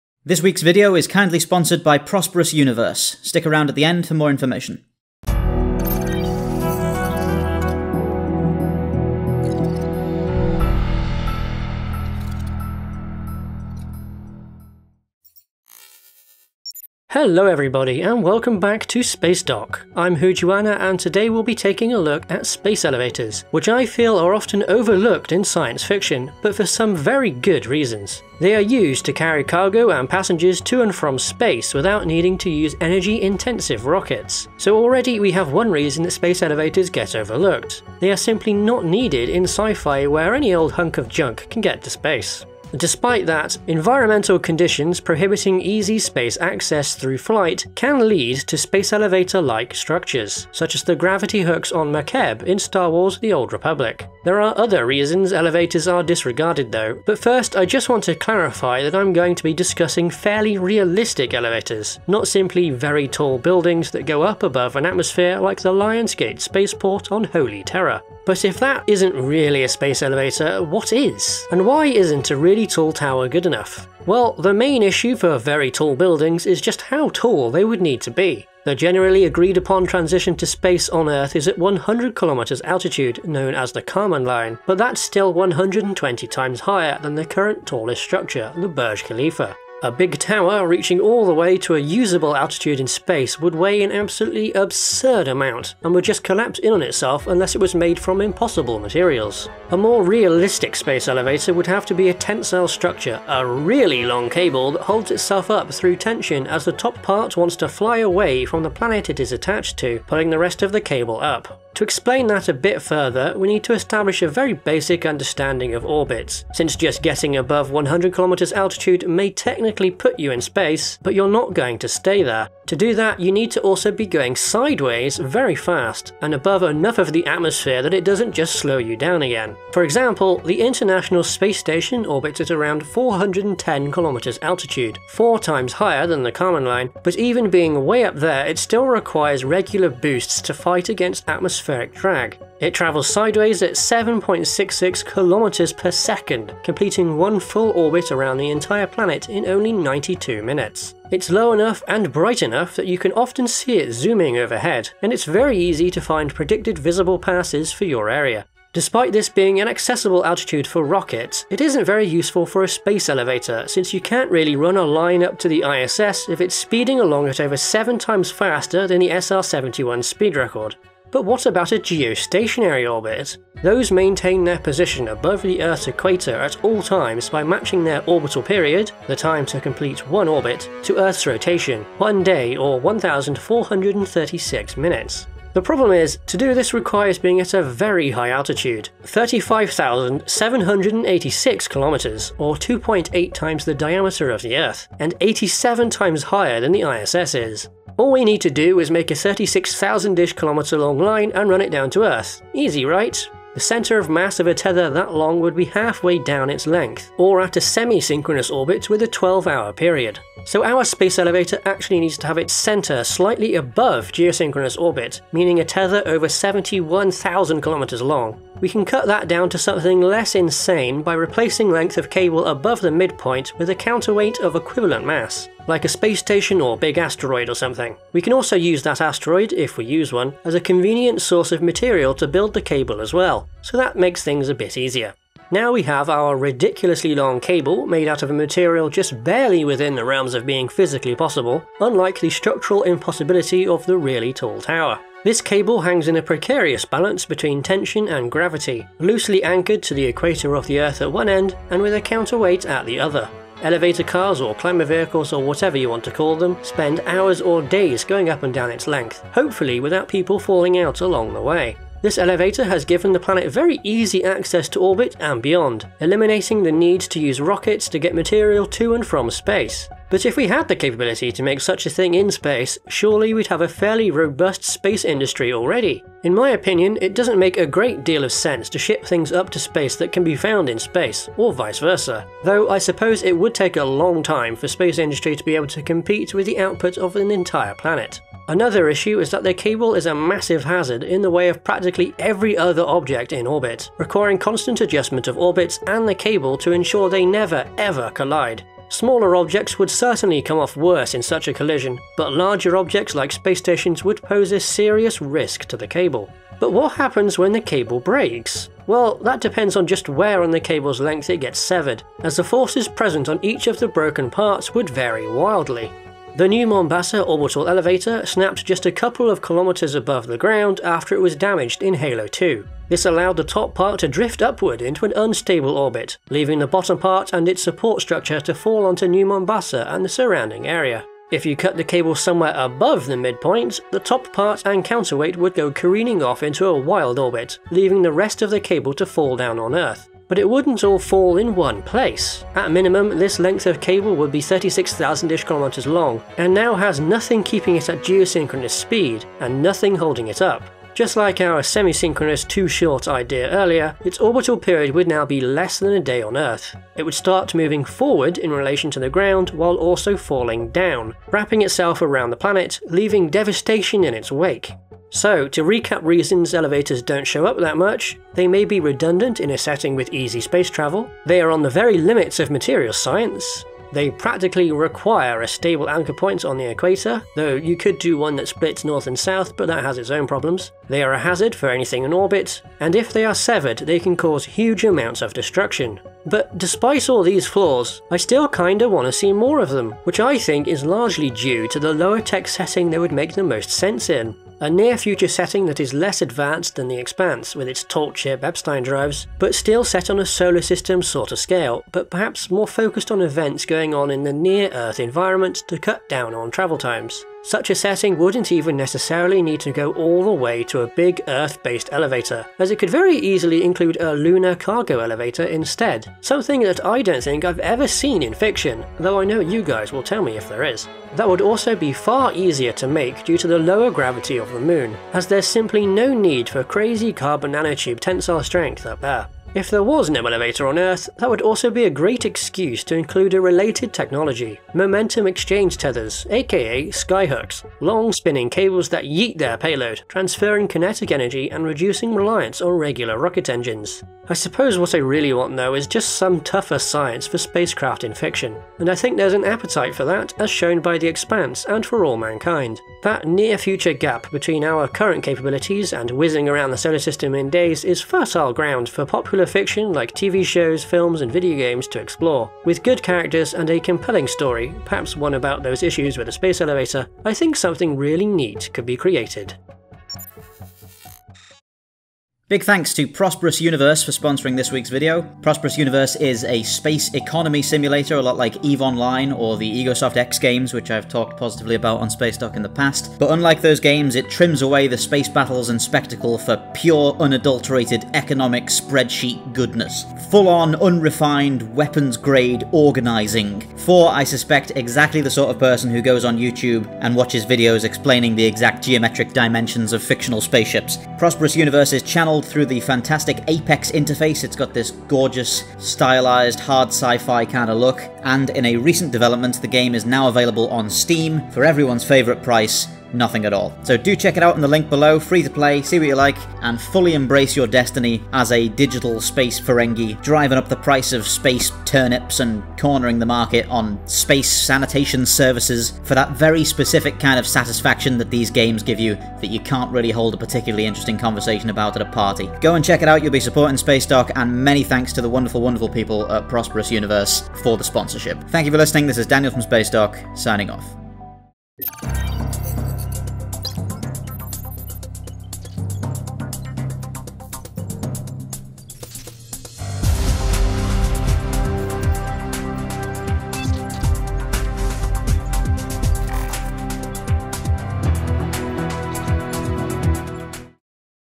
Space Elevators in Science Fiction
Check out Prosperous Universe with this link and support Spacedock!

Spacedock delves into the fascinating but flawed concept of Space Elevators.